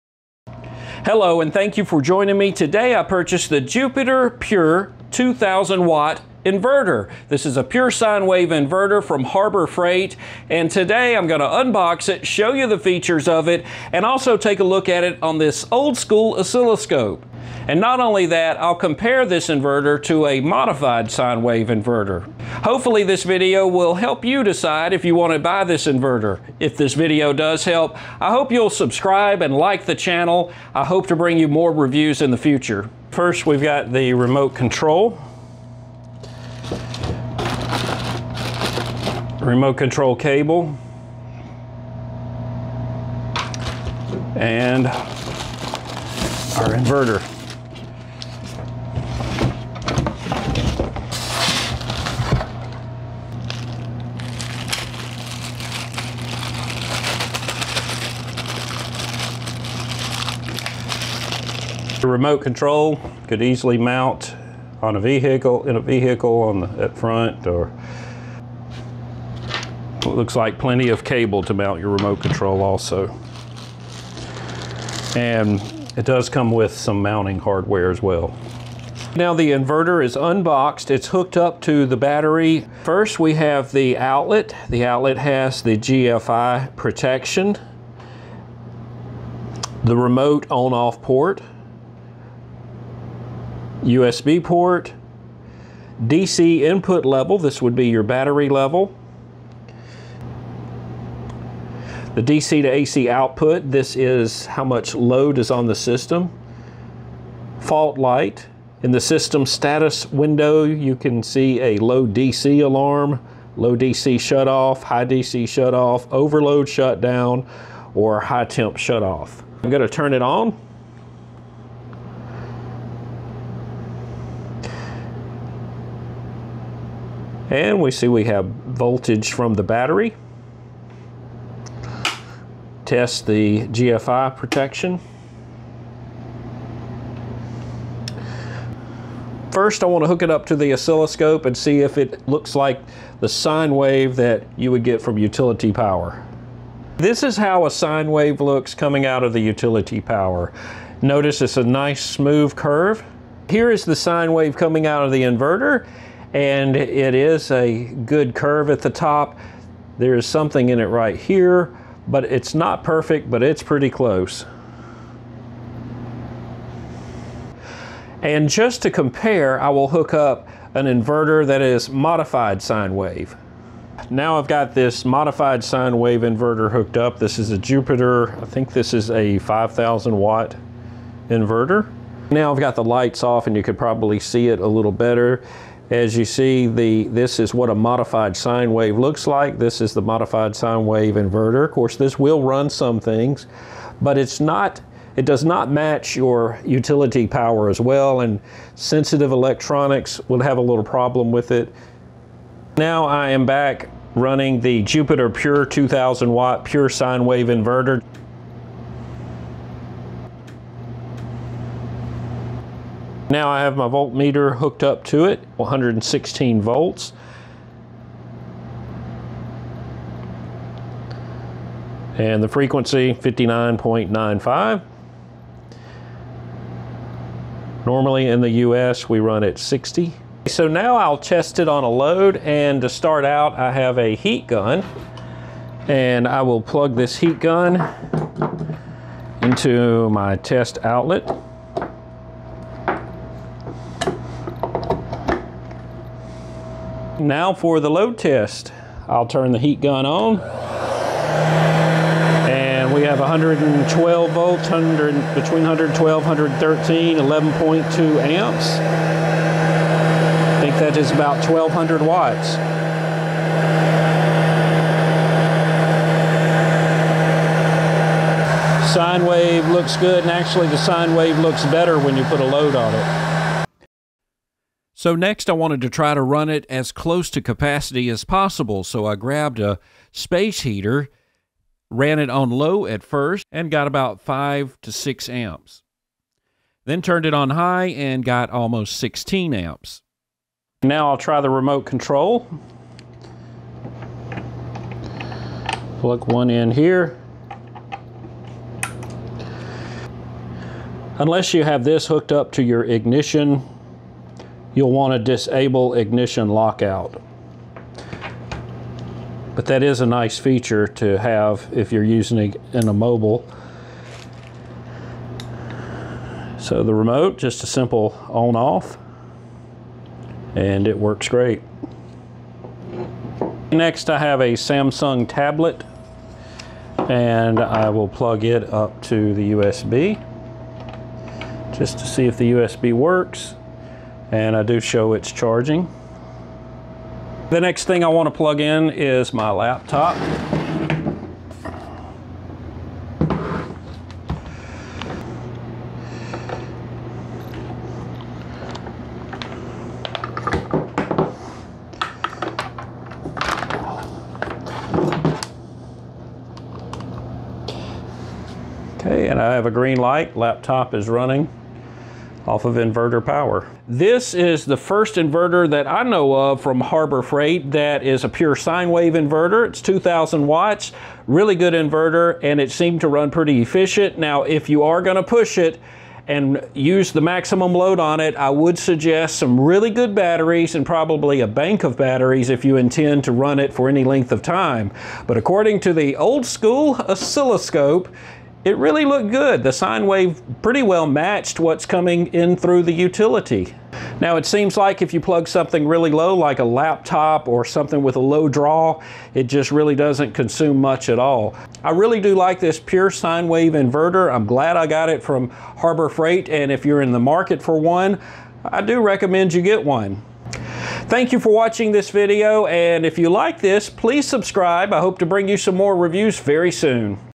Pure Sine Wave Power Inverter Review.
Jupiter Pure™ 2000 watt pure sine wave power inverter

In this review, I'll take you through the unboxing and run this inverter on a full load. Testing the inverter using an oscilloscope, volt meter and amp meter. I'll also give you my opinion of how it performs and let you decide whether or not it's the right choice for your needs.

If you're in the market for a power inverter, be sure to watch this video.

Amazon link for test equipment used in this video:
Fluke 233 True-RMS Volt Meter

Fluke 324 True-RMS Clamp Meter